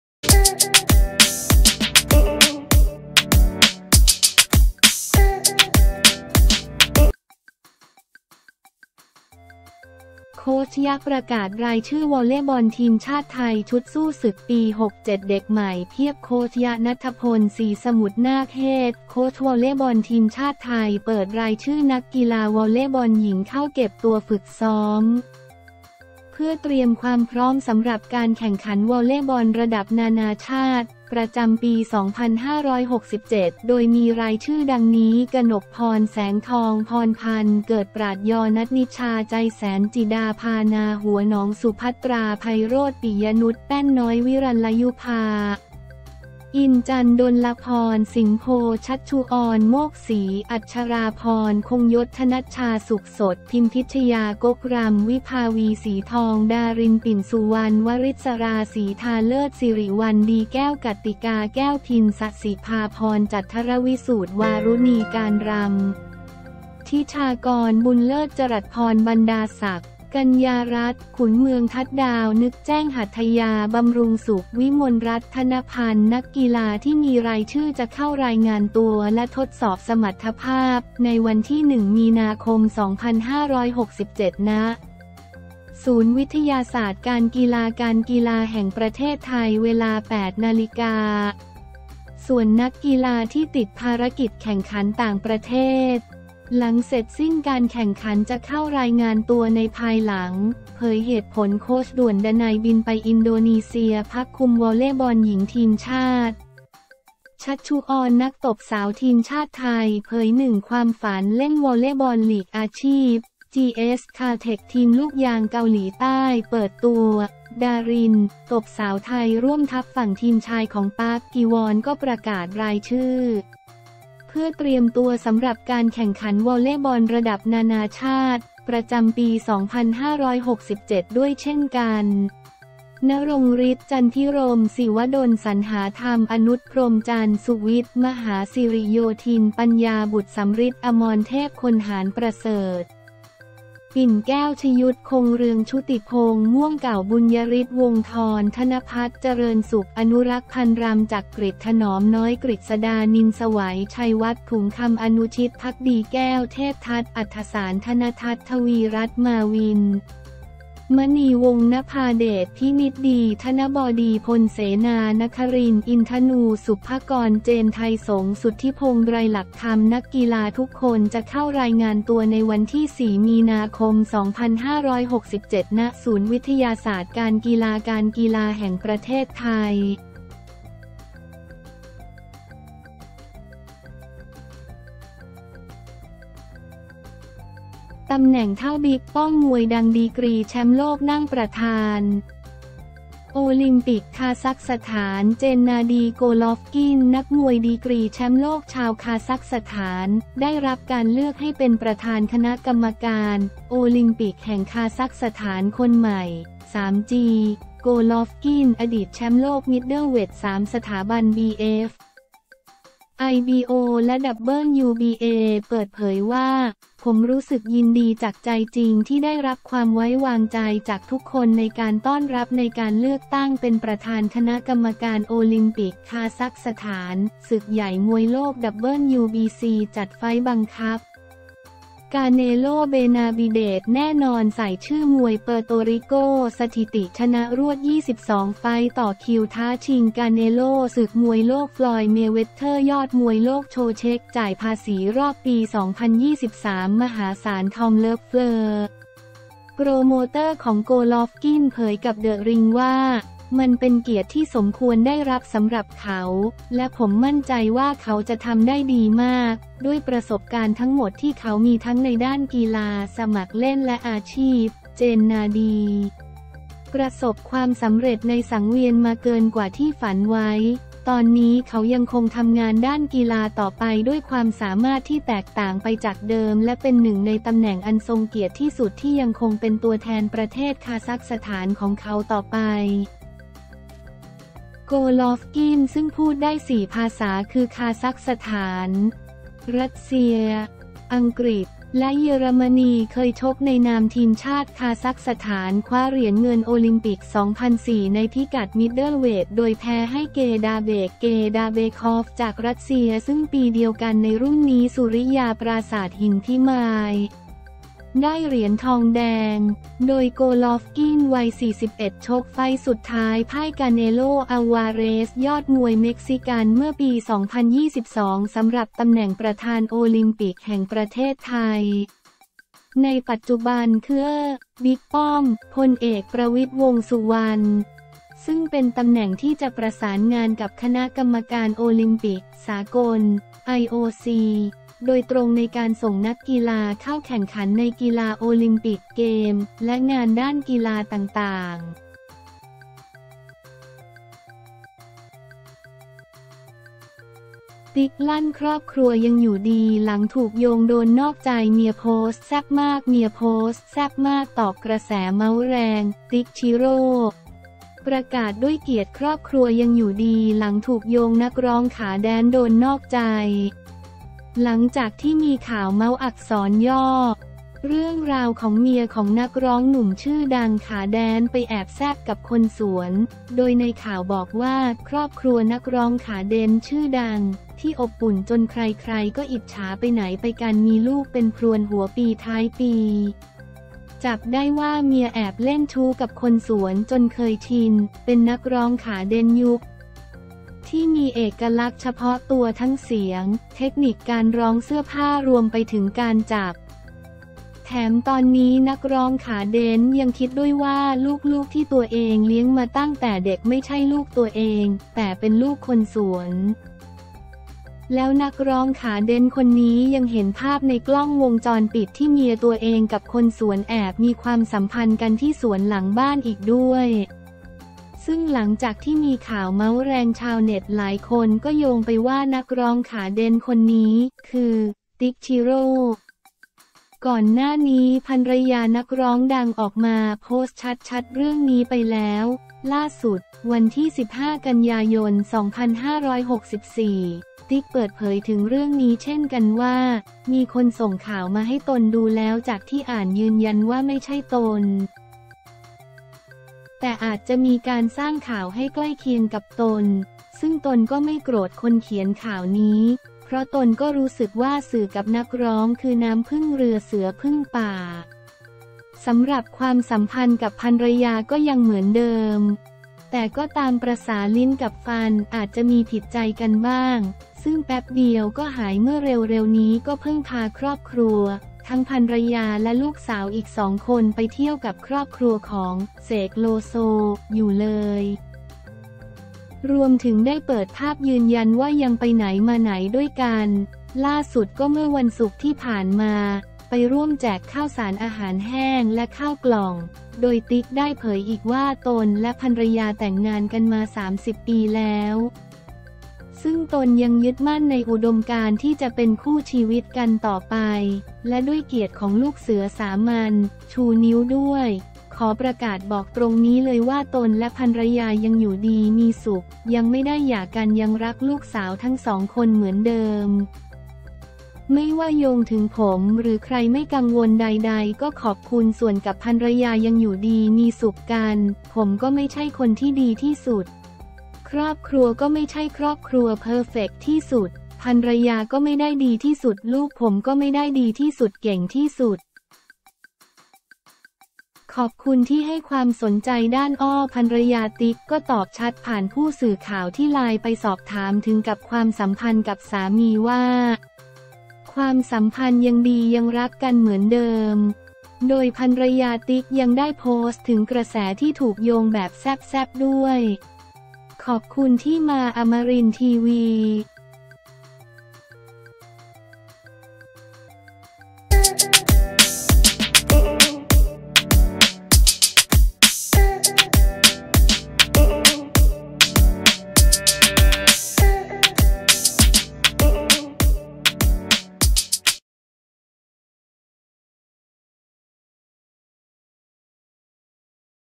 โค้ชยะ ประกาศรายชื่อ วอลเลย์บอล ทีมชาติไทย ชุดสู้ศึกปี 67 เด็กใหม่เพียบ
โค้ชยะ ณัฐพนธ์ ศรีสมุทรนาค เฮดโค้ช วอลเลย์บอล ทีมชาติไทย เปิดรายชื่อนักกีฬาวอลเลย์บอลหญิงเข้าเก็บตัวฝึกซ้อม เพื่อเตรียมความพร้อมสำหรับการแข่งขันวอลเลย์บอลระดับนานาชาติ ประจำปี 2567 โดยมีรายชื่อดังนี้

กนกพร แสงทอง
พรพรรณ เกิดปราช์ญ
ณัฐณิชา ใจแสน
จิดาภา นาหัวหนอง
สุพัตรา ไพโรจน์
ปิยะนุช แป้นน้อย
วิรัลยุพา อินจันทร์
ดลพร สินโพธ์
ชัชชุอร โมกศรี
อัจฉราพร คงยศ
ธนัชชา สุขสด
พิมพิชยา ก๊กรัมย์
วิภาวี ศรีทอง
ดาริน ปิ่นสุวรรณ
วริศรา สีทาเลิศ
สิริวัลย์ ดีแก้ว
กัตติกา แก้วพิน
ศศิภาพร จัทรวิสูตร
วารุณี การรัมย์
ฑิชากร บุญเลิศ
จรัสพร บรรดาศักดิ์
กัญญารัตน์ ขุนเมือง
ทัดดาว นึกแจ้ง
หัตถยา บำรุงสุข
วิมลรัตน์ ทะนะพันธุ์
นักกีฬาที่มีรายชื่อจะเข้ารายงานตัวและทดสอบสมรรถภาพ ในวันที่ 1 มีนาคม 2567 ณ ศูนย์วิทยาศาสตร์การกีฬา การกีฬาแห่งประเทศไทย เวลา 08.00 น. ส่วนนักกีฬาที่ติดภารกิจแข่งขันต่างประเทศ หลังเสร็จสิ้นการแข่งขันจะเข้ารายงานตัวในภายหลัง

เผยเหตุผล โค้ชด่วน ดนัย บินไปอินโดนีเซีย – พักคุม วอลเลย์บอลหญิง ทีมชาติ?
ชัชชุอร นักตบสาวทีมชาติไทย เผย1ความฝันเล่นวอลเลย์บอลลีกอาชีพ
จีเอส คาลเท็กซ์ ทีมลูกยางเกาหลีใต้ เปิดตัว ‘ดาริน’ ตบสาวไทยร่วมทัพ
ฝั่งทีมชายของ ปาร์ก กี วอน ก็ประกาศรายชื่อ เพื่อเตรียมตัวสำหรับการแข่งขันวอลเลย์บอลระดับนานาชาติ ประจำปี 2567 ด้วยเช่นกัน
ณรงค์ฤทธิ์ จันภิรมย์
ศิวดล สันหาธรรม
อนุตร พรมจันทร์
สุวิทย์ มหาสิริโยธิน
ปัญญา บุตรสัมฤทธิ์
อมรเทพ คนหาญ
ประเสริฐ ปิ่นแก้ว
ชยุตม์ คงเรือง
ชุติพงศ์ ม่วงเก่า
บุญญฤทธิ์ วงศ์ธร
ธนภัทร์ เจริญสุข
อนุรักษ์ พันธุ์รัมย์
จักกริช ถนอมน้อย
กฤษฎา นิลไสว
ชัยวัฒน์ ถุงคำ
อนุชิต ภักดีแก้ว
เทพทัต อรรถสาร
ธนทัต ทวีรัตน์
มาวิน มณีวงษ์
นภาเดช พินิจดี
ธนบดี พลเสนา
นครินทร์ อินธนู
ศุภกร เจนไธสง
สุทธิพงษ์ ดรหลักคำ

นักกีฬาทุกคนจะเข้ารายงานตัวในวันที่ 4 มีนาคม 2567 ณ ศูนย์วิทยาศาสตร์การกีฬา การกีฬาแห่งประเทศไทย